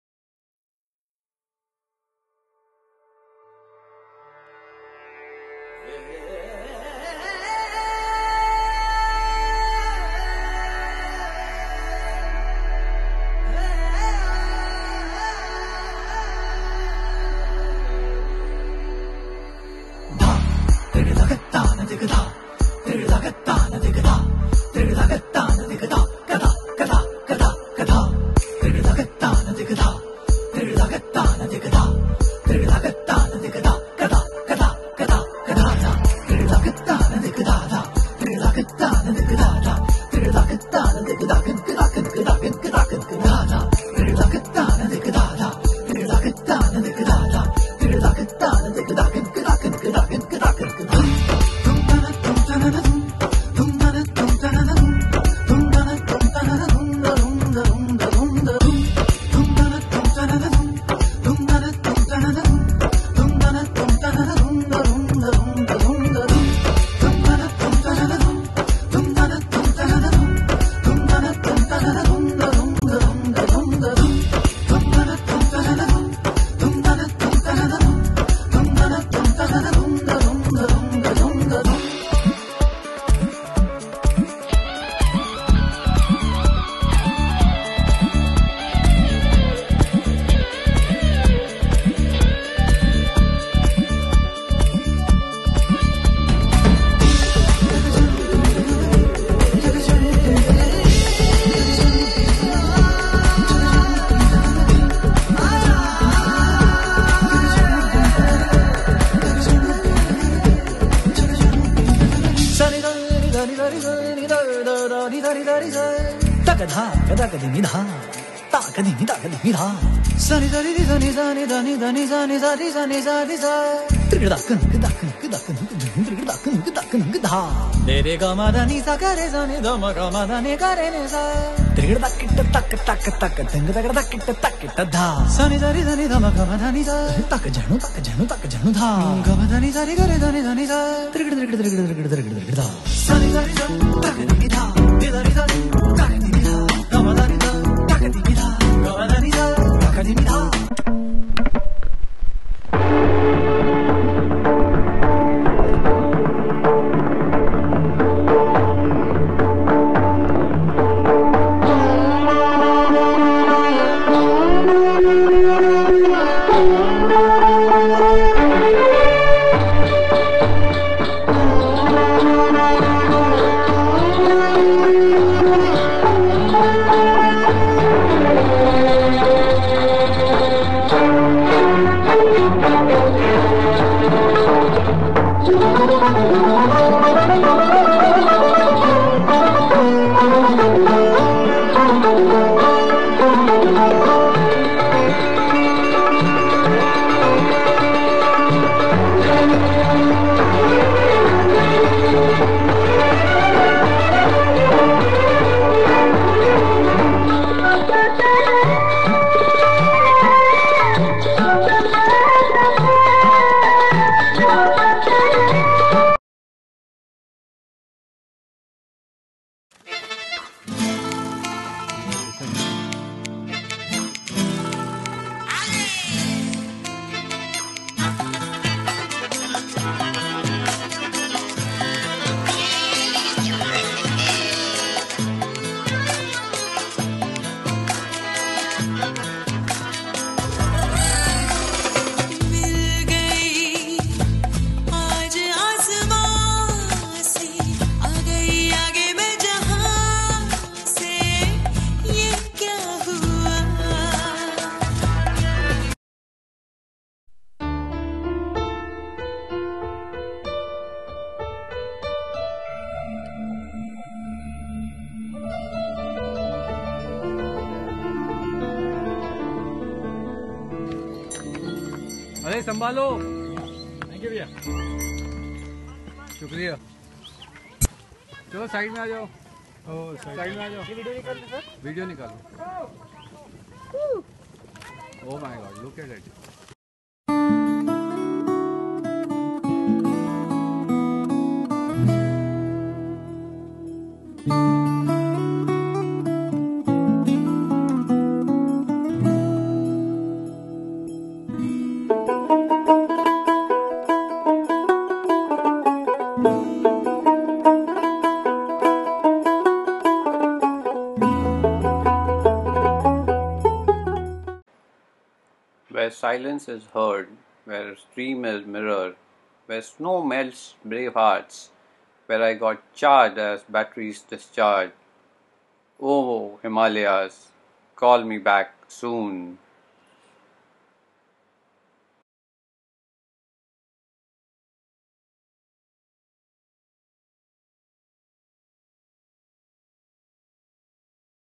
6 day Himalayan Trek - Kedarkanth , Uttarkashi.
12500 feet kedarkanth trek from Dehradun - Gaichawan Gaon - Jalouta - Pukhrola - Kedarkanth - Taj - Gaichawan gaon. 6 days. 0 degree temperature.
Where Silence is Heard,
Where Stream is mirror,
Where Snow melts bravehearts
Where I got charged as batteries drained out.
Oh! Himalayas .call me back soon.